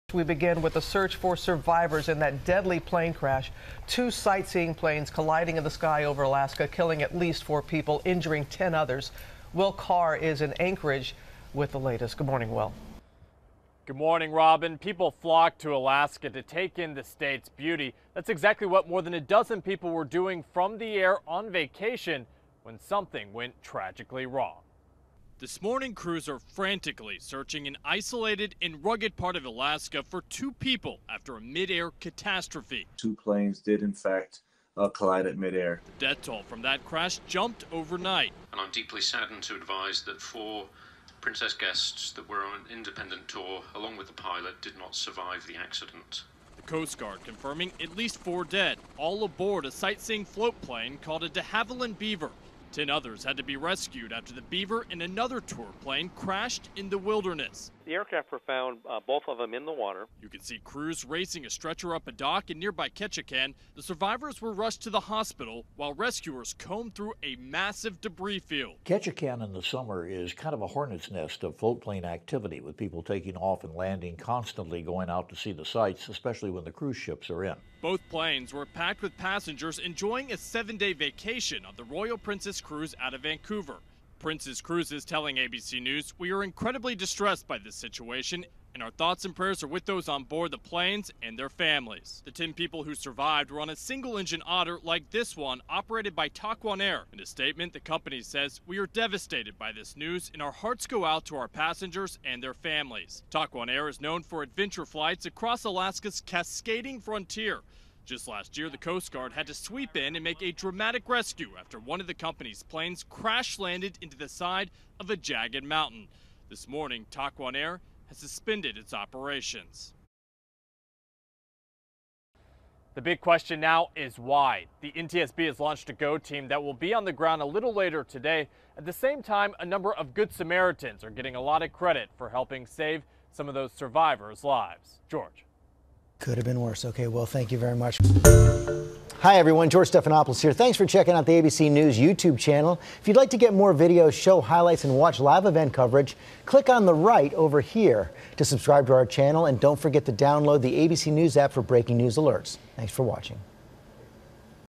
At Least Four People Dead After Two Aircraft Collide Over Alaska
May 14, 2019 - Two people are still missing and 10 others hospitalized after the deadly collision in Coon Cove, Alaska, according to officials.